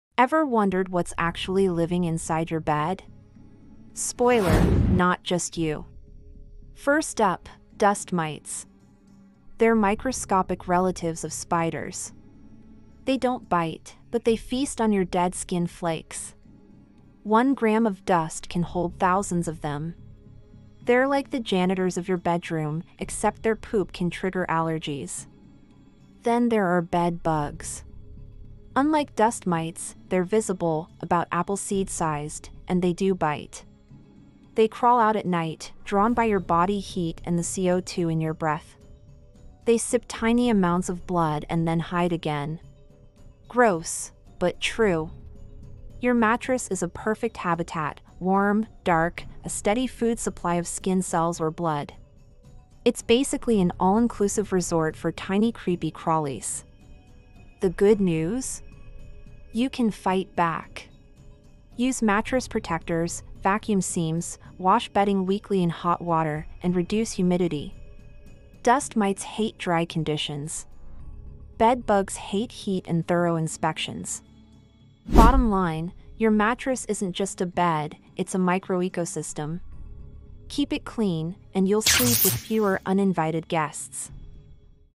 The Tiny Monsters Living in Your Mattress 🛏️😱
Dust mites and bed bugs thrive where you sleep — warm, dark mattresses full of skin flakes. Learn how these tiny creatures live, why they love your bed, and how to kick them out with simple cleaning habits.